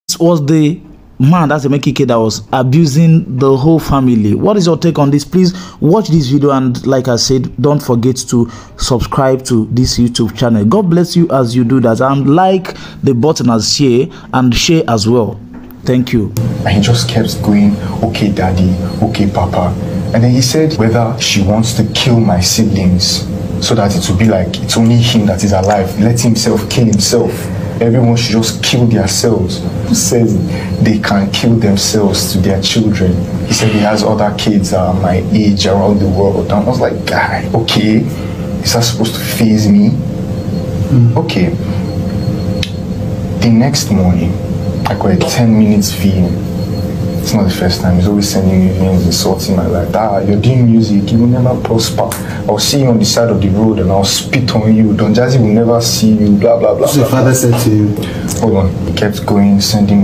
Will Emeka Ike and Don Jazzy have problem because of this video?
Suzanne Emma, the ex-wife of veteran Nollywood Actor, Emeka Ike, has revealed that all that her ex-husband said in a recent interview about her and their marriage are nothing but lies.

Emeka Ike had recently revealed that he lost everything he ever owned to his wife over allegations of domestic abuse.
This is coming even as the son also shared some secrets.